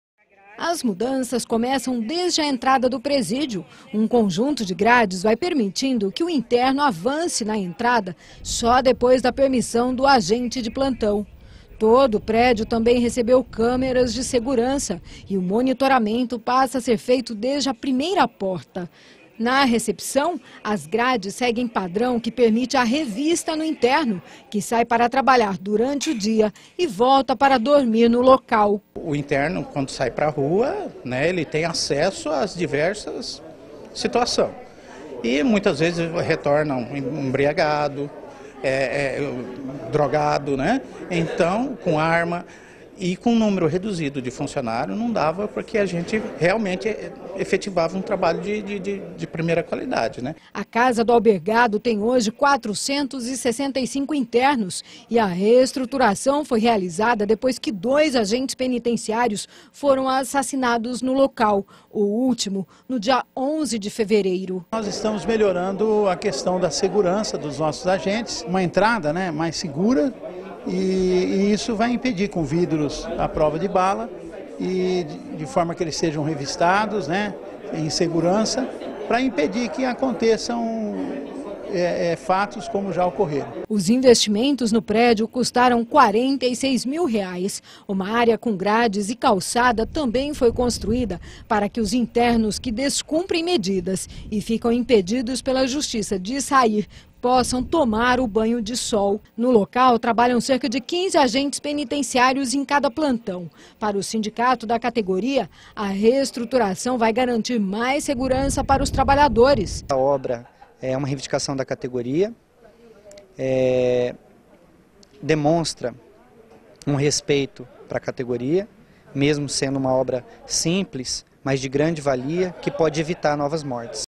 Casa do Albergado recebe melhorias para garantir segurança de agentes
Após o assassinato de dois agentes penitenciários, um em 2011 e outro neste ano, na Casa do Albergado, na Vila Sobrinho, na Capital, agora o local passou por reestruturação para dar mais segurança aos trabalhadores e também aos internos.